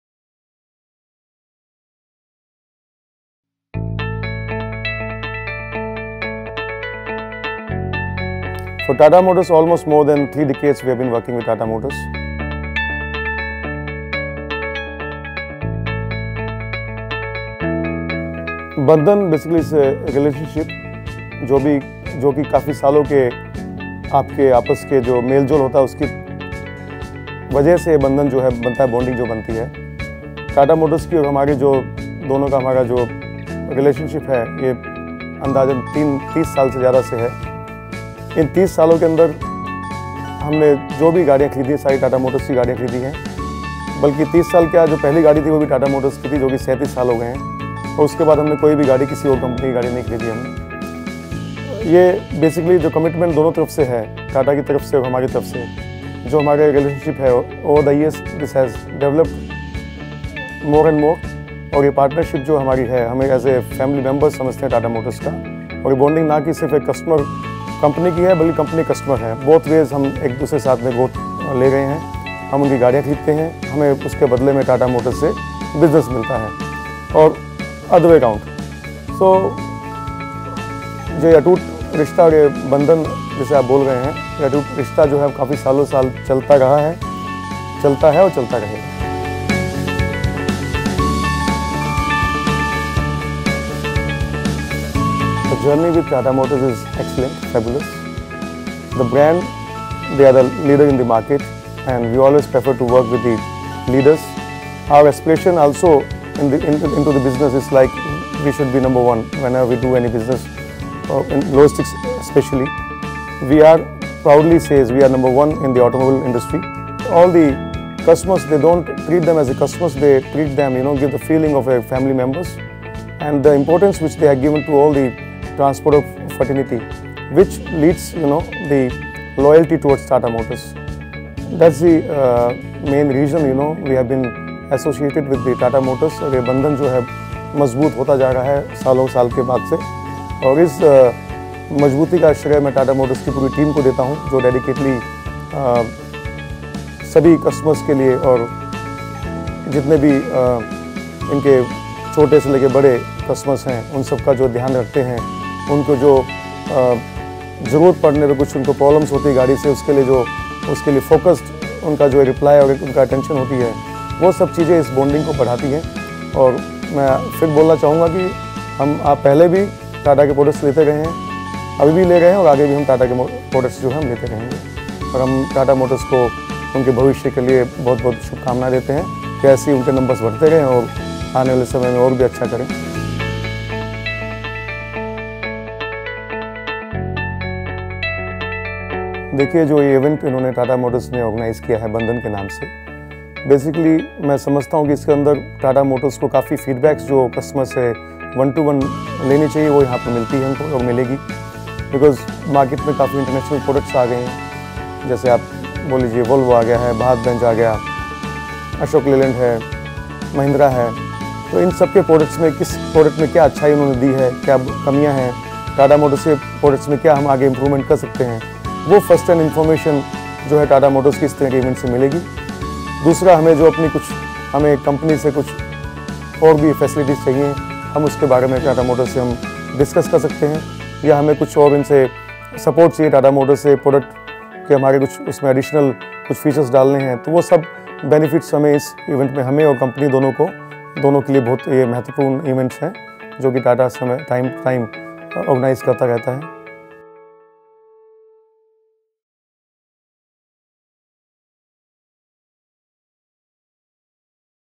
Tata Motors Bandhan: Mukesh Haritash, Chetak Logistics
“Tata Motors has always treated us more like their family members than customers. I congratulate the entire team at Tata Motors for making this bond grow stronger with the passage of time.”